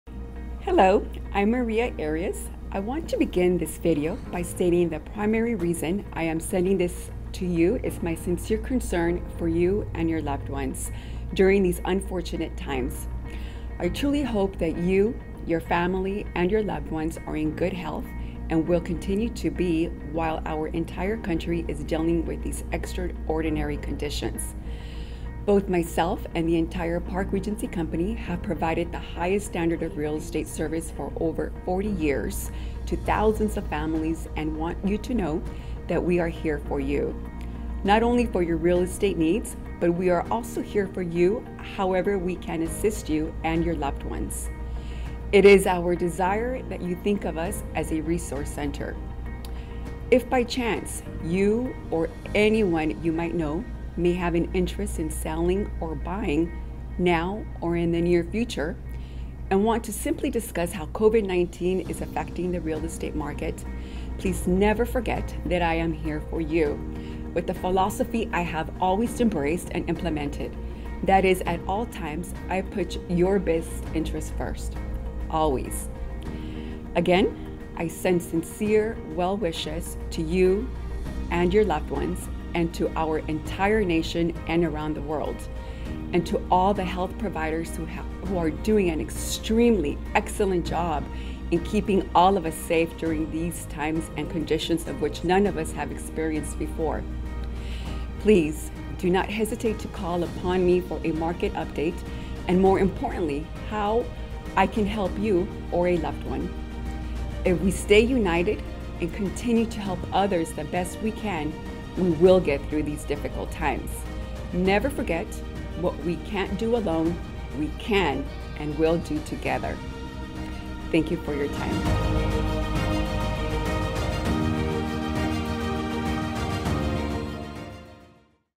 Covid-19 Real Estate Resource
As a concerned active Realtor in your community I want to let the public know that I'm here for you to assist in any Covid-19 resources you may need or any Real Estate needs in buying or selling.